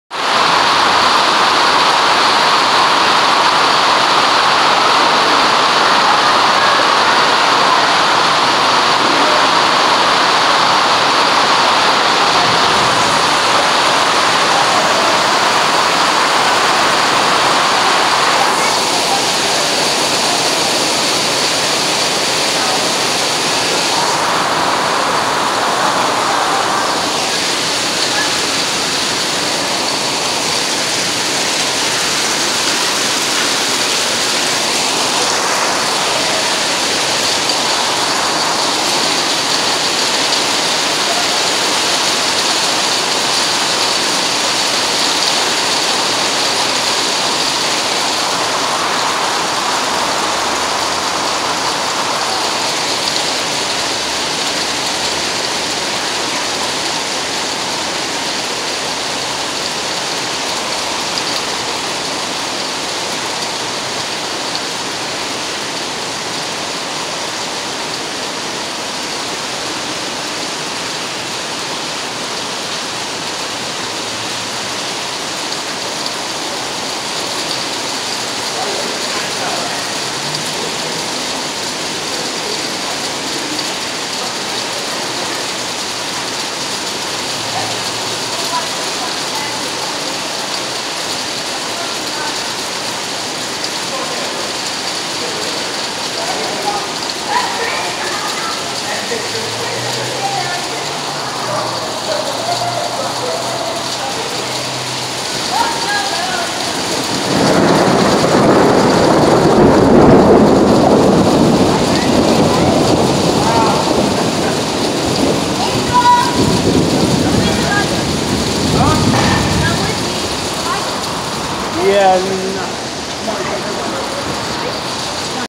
Rain and Thunder in Sacramento [Natomas High School]
This video was captured when I was attending Natomas High. This is what I consider to be perfect weather for sleeping in. It doesn't really rain much in Sacramento. I found this video on my old phone and it was originally recorded on 04/16/2018. Also sorry for the vertical recording :)